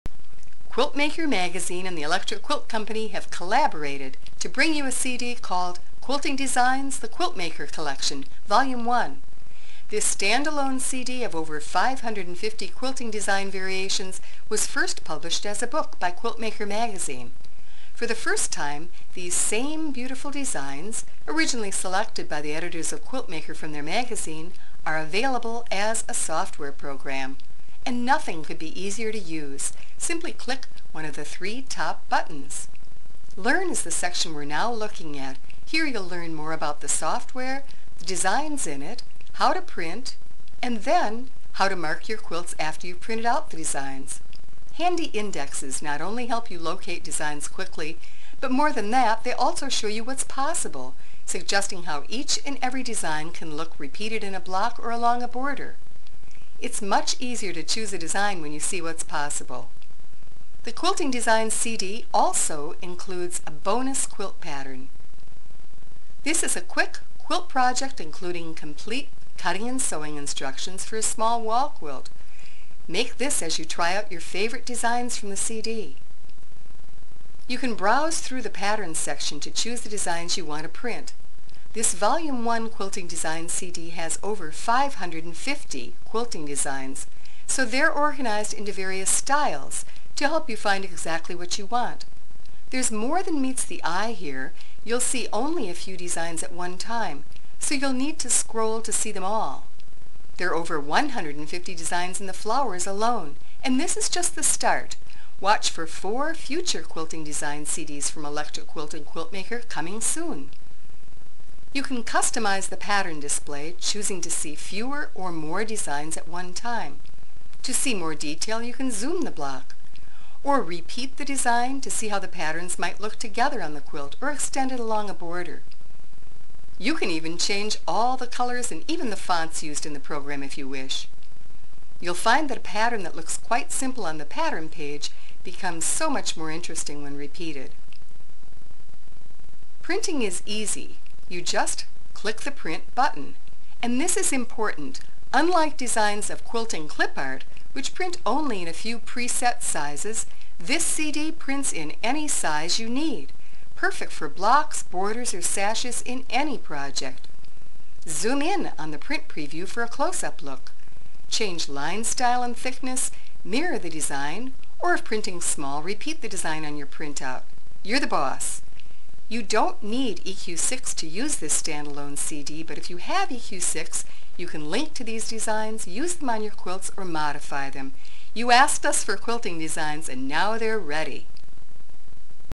The Quiltmaker Collection Sample CD "How To" Video (CD-Rom 2007)
another random disc i found for free at a used art supply store